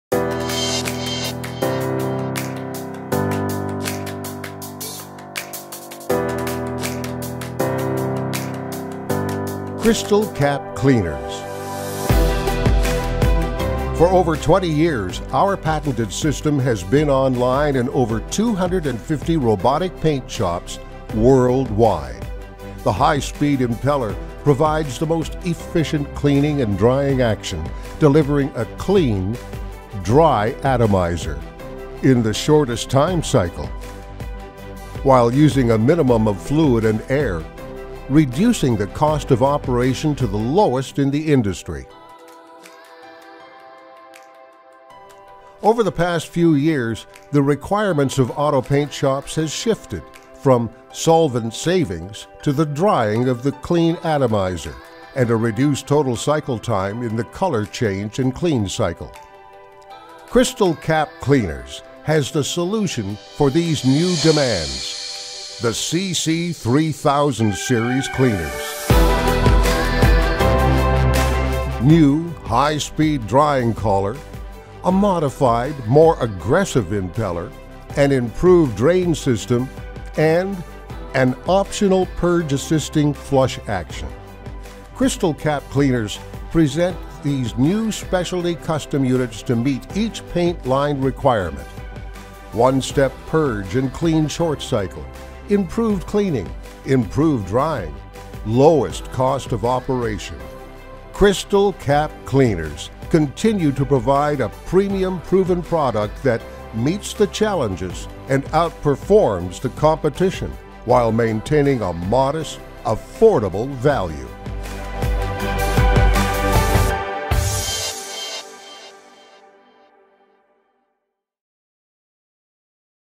CC 3000 Series Robotic Paint Atomizer Cleaner
The all new CC-3000 Series Crystal cap Cleaner redesigned from top to bottom that incorporates:
- a new mounting base
- a new drain assembly
- improved internal color changes
- captive lid fasteners
- a more efficient impellor
- new sealing and high-speed drying collar
- and a super slip finish interior.

Benefits:
- improved cleaning in less time
- rapid drying cycle
- less overall cycle time
- efficient color changes in unit
- fluid and air savings
- more efficient air/fluid separation
- lowest cost of operation

The new CC-3000 remains the BEST VALUE in robotic paint atomizer cleaners.